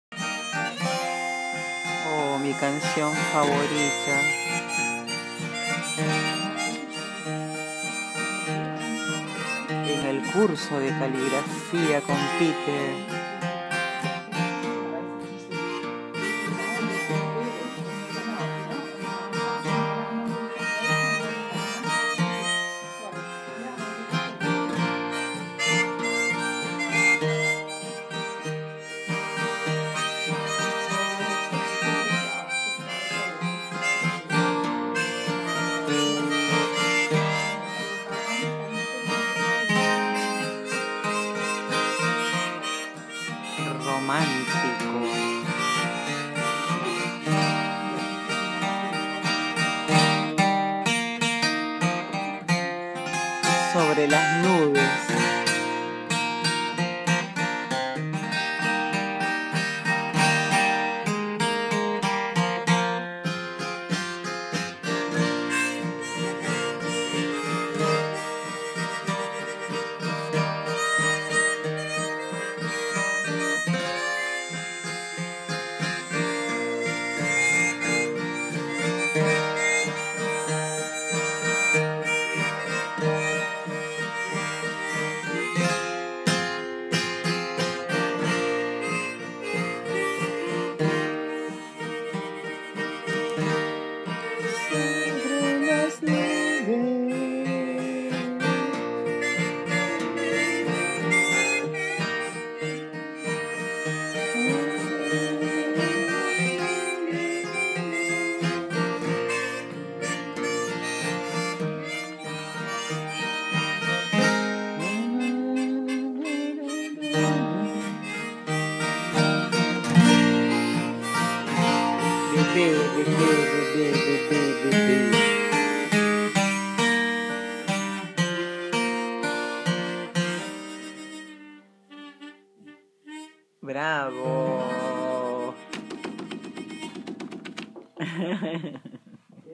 Curso de Caligrafia día 2
Peter tocando mi canción favorita escribiendo la letra B mi bebé